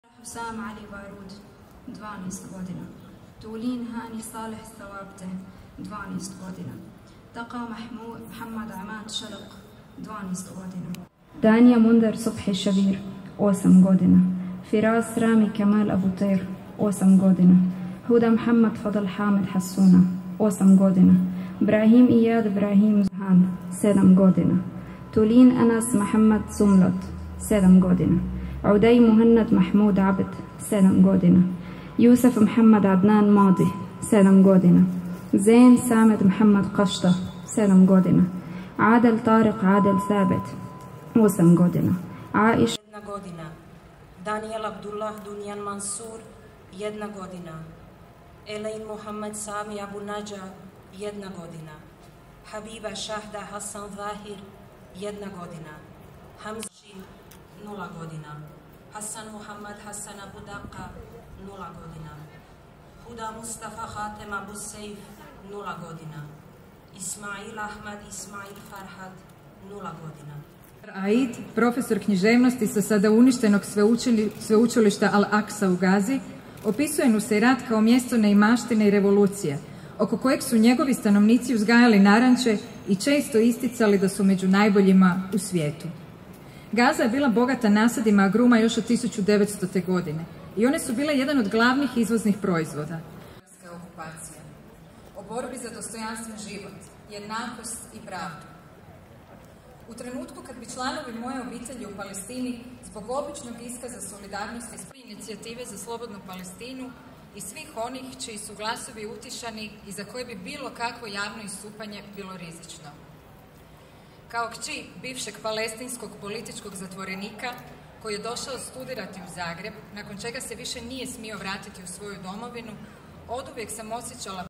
Prosvjedna akcija Inicijative za slobodnu Palestinu
Okupljanje Inicijative za slobodnu Palestinu na Europskom trgu povodom Svjetskog dana izbjeglica. Format okupljanja je tzv. "die-in" odnosno prosvjedna akcija u kojoj sudionici i sudionice leže na podu i tako predstavljaju ubijene u Gazi i odaju im počast.